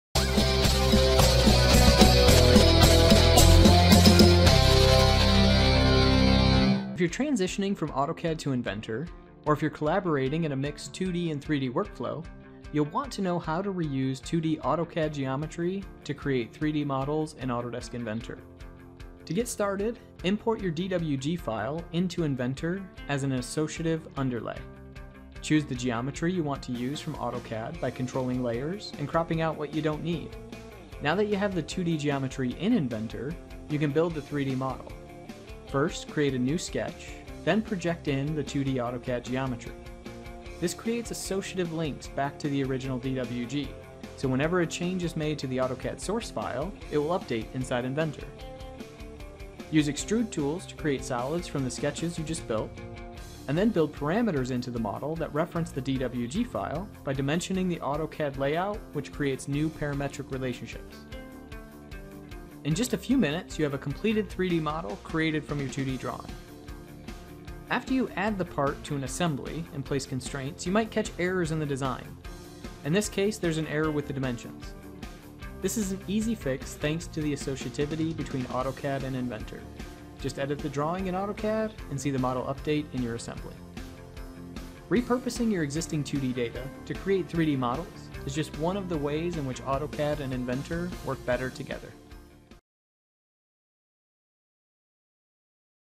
2D-3D | Kết hợp giữa thiết kế 2D và 3D trong cơ khí
(Video có hỗ trợ phụ đề tiếng Việt)
Sự liên kết giữa bản vẽ 2D và mô hình 3D là tính năng gần như là một trong những tính năng độc đáo nhất của Autodesk với bộ đôi AutoCAD và Inventor, điều này giúp quy trình làm việc được đồng bộ và trở nên nhanh chóng hơn.

Hotline tư vấn giải pháp triển khai phần mềm Autodesk (cho doanh nghiệp)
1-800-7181